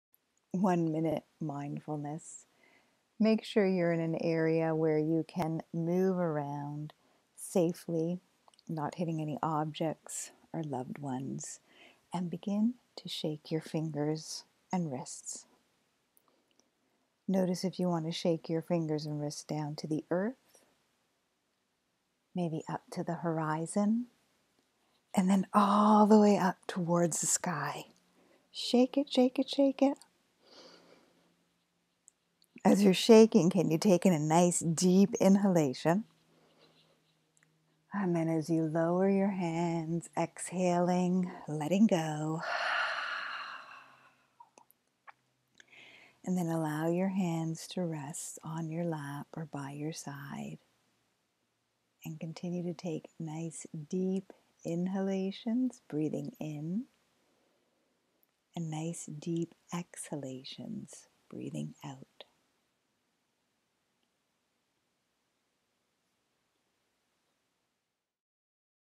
OMM One Minute Mindfulness - Shake it!! Made for kids of all ages.
One Minute Mindfulness  breath awareness. Reduce stress and anxiety, calm your nervous system. Balance your emotions, have fun, be here now. Made for kids of all ages!

Photo taken at Kripalu Center for Yoga and Health  -Lake Mahkeenac also known as The Stockbridge Bowl,  Massachusetts